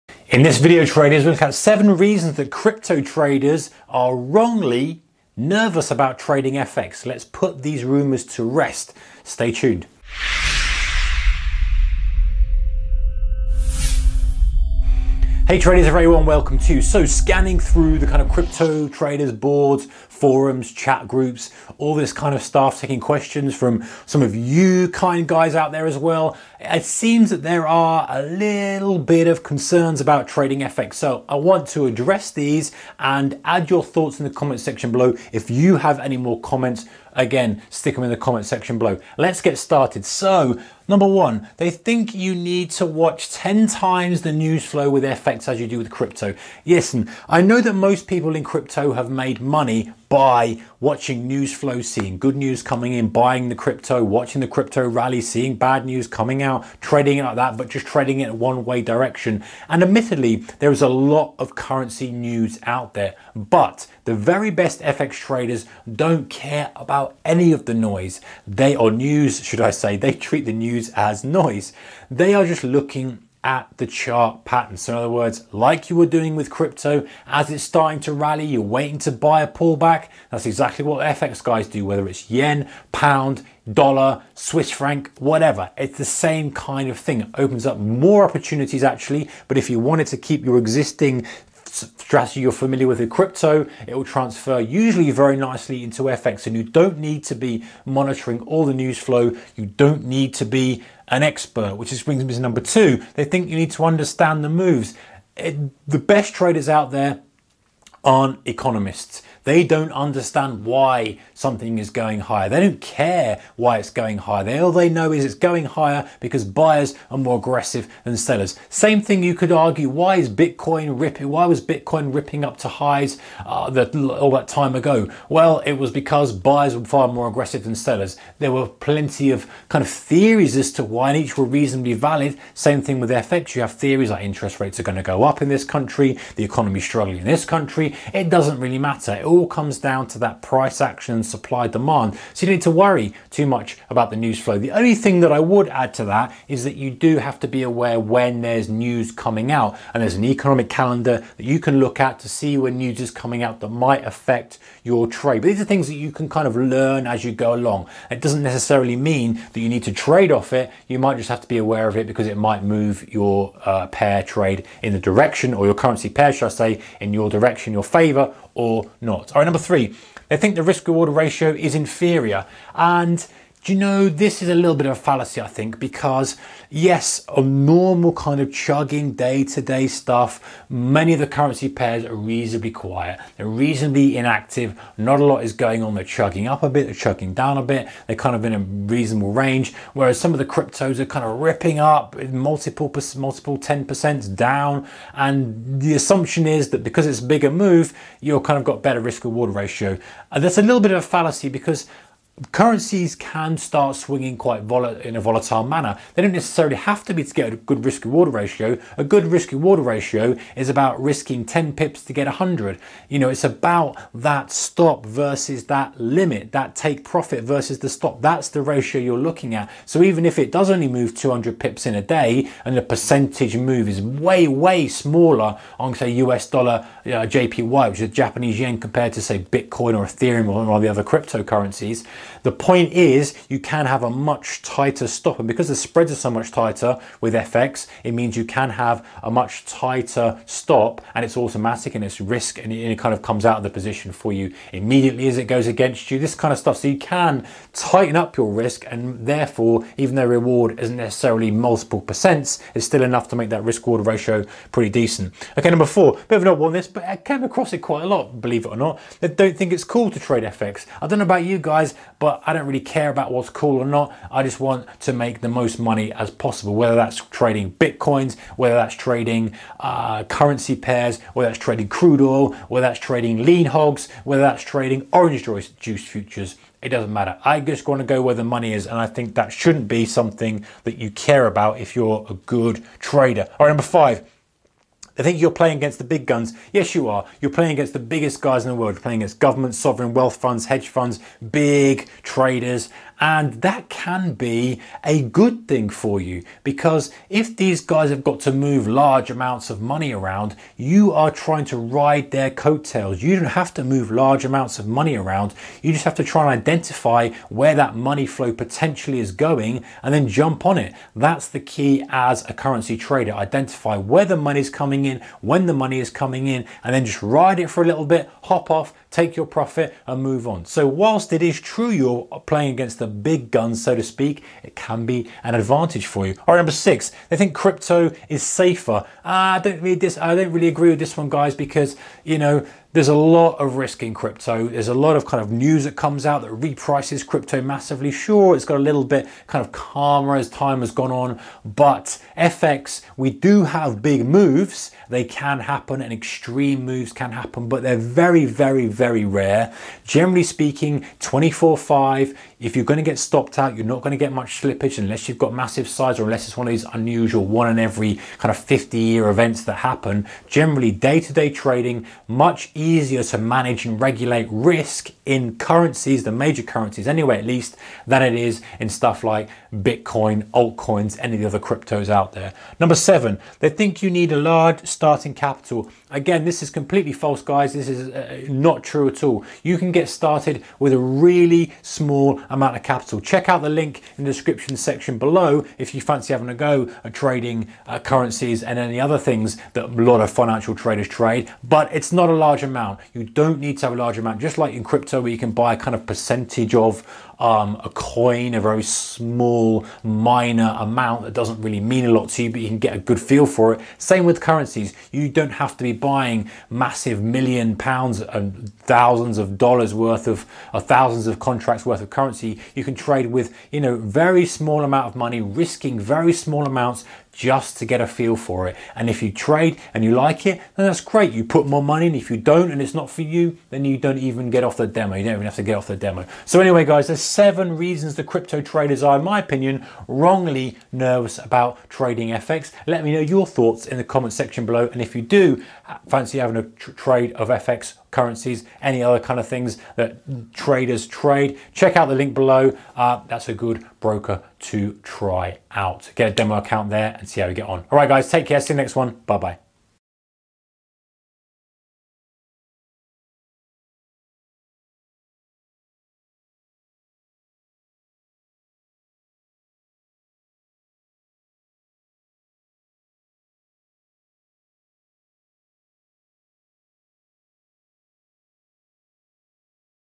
7 Reasons Crypto Traders are Wrongly Nervous about Trading FX ☝
★ Reasons Crypto Traders are Wrongly Nervous about Trading FX

7 reasons Crypto traders are wrongly nervous about trading FX.
- They think you need to watch 10 times the newsflow as much as crypto
-  They think you need to understand the moves.
-  They think the risk/reward ratio is inferior.
-  They think forex trading is not cool
-  They think you are playing against big guns.  Yes, you are but that isn't necessarily a bad thing.
-  They think crypto is safer
- They think you need a large starting capital